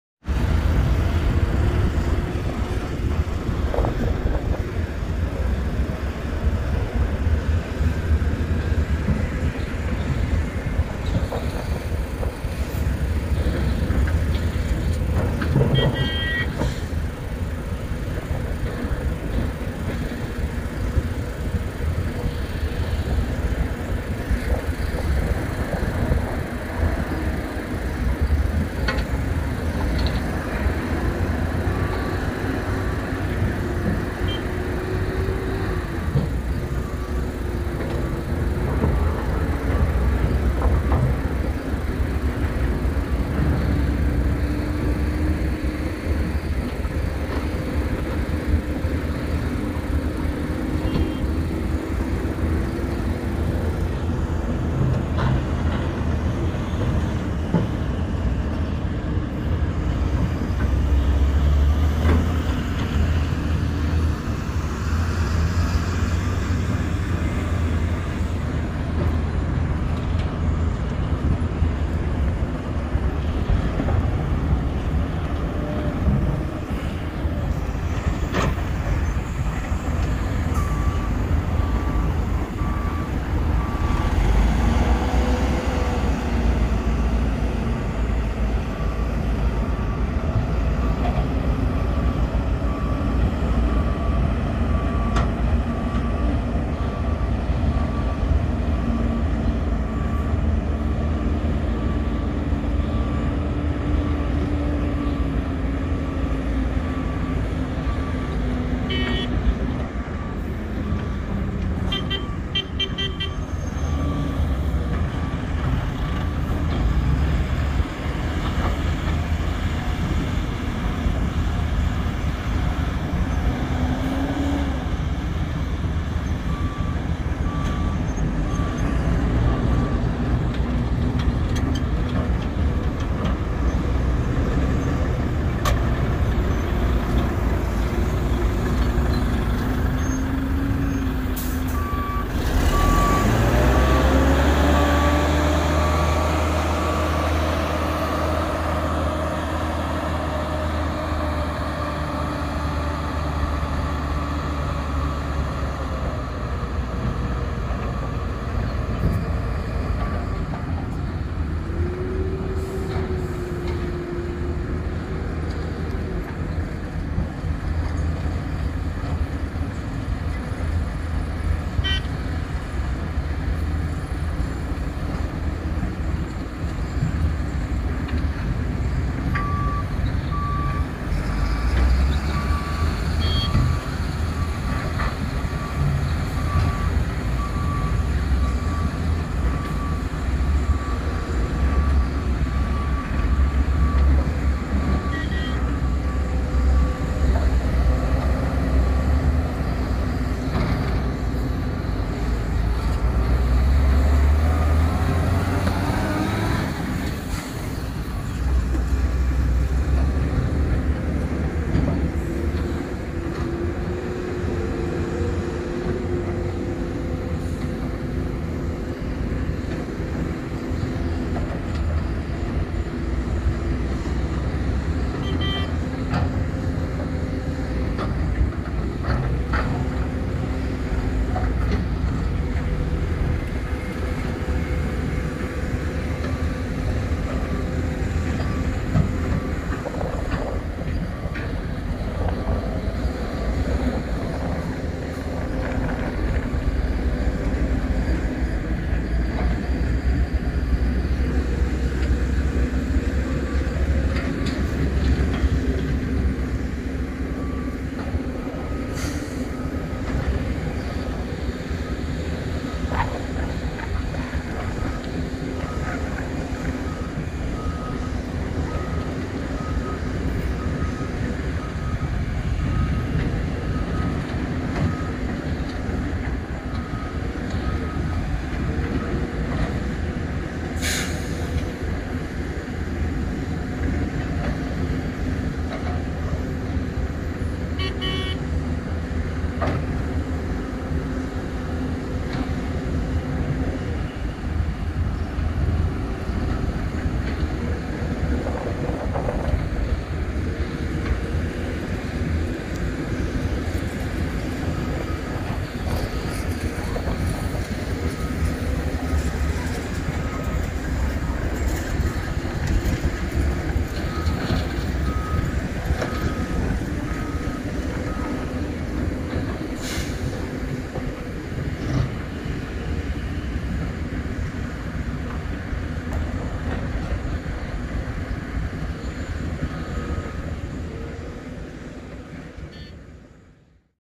Hitachi Zaxis 470H Excavators Loading Volvo And Eicher Trucks On The Line in Mines
Amazing Excavator at Work With Trucks, Dumpers, loader, Dozer, Grader All Attachment working in mud, soil, coal in mines.
Brand: BEML, Volvo, Hitachi, Kobelco, Scania, Hyundai, Eicher, CAT, Komatsu,

Appropriate content for Entertainment.